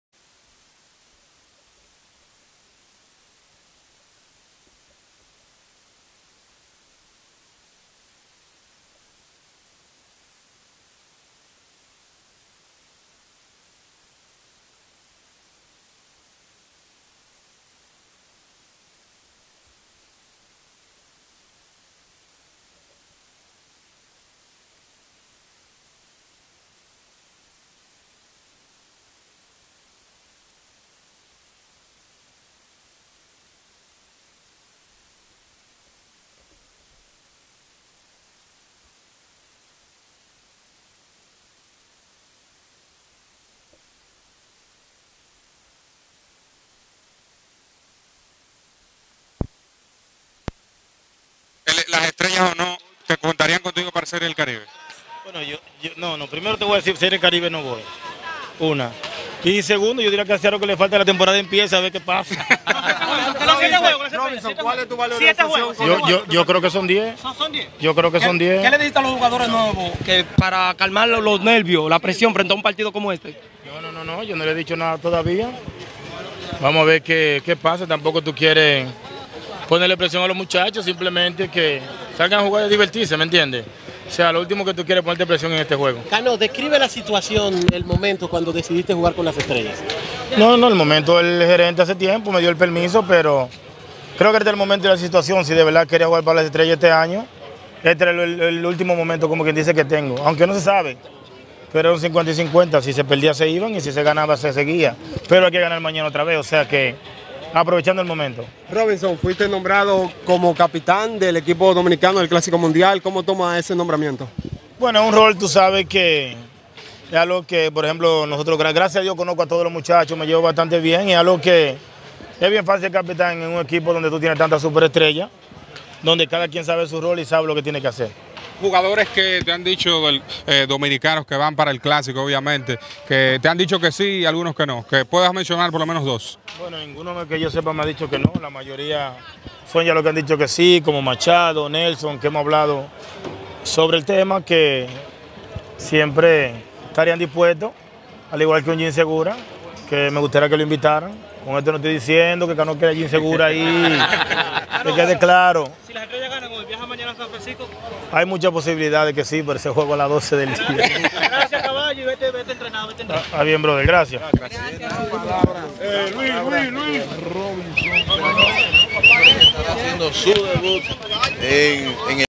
Entrevista Robinson Cano
Entrevista a Robinson cano antes del juego contra los Leones del Escogido 22 diciembre 2016.